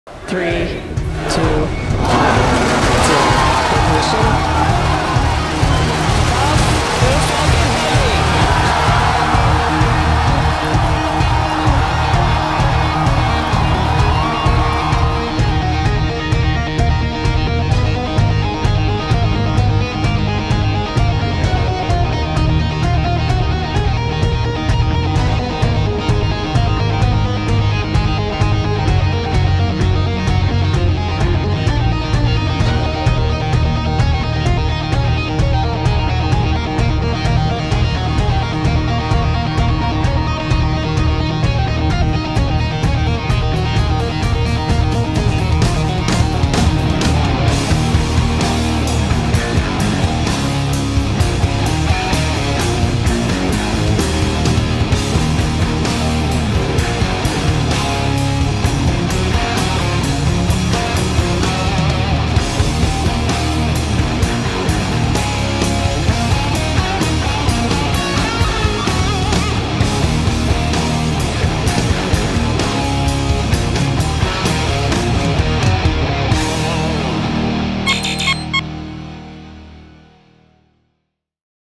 Starman’s Flight into Deep Space - Falcon Heavy Launch to 500K Miles Away
On Feb 6, SpaceX made history with the maiden flight of the Falcon Heavy rocket and Tesla Roadster payload. The Virtual Telescope Project, Tenagra Observatories and astrophotographer Rogelio Bernal Andreo have captured imagery of the Roadster and its mannequin driver “Starman” in flight.

Credit: Space.com / Produced and Edited by Steve Spaleta
Footage courtesy SpaceX,  Gianluca Masi (Virtual Telescope Project) and Michael Schwartz (Tenagra Observatory) and Rogelio Bernal Andreo (DeepSkyColors.com)